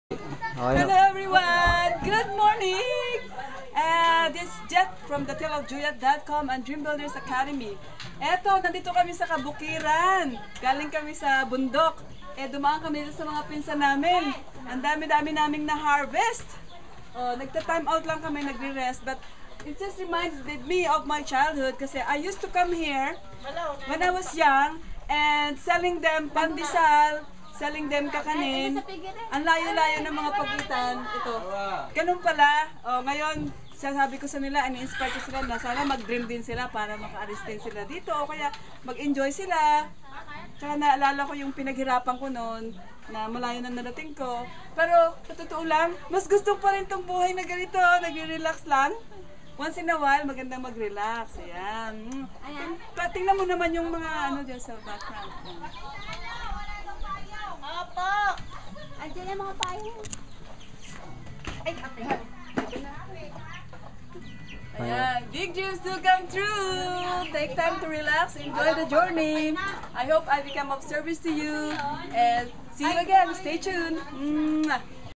Little Blasts from the Past Part 1
Jhet, after climbing the mountain, takes a rest and shares some of her childhood experiences -- her little blasts from the past. She realizes how far she had gone and is now ready to inspire more people.

What are your childhood experiences that helped you grow? Share them below.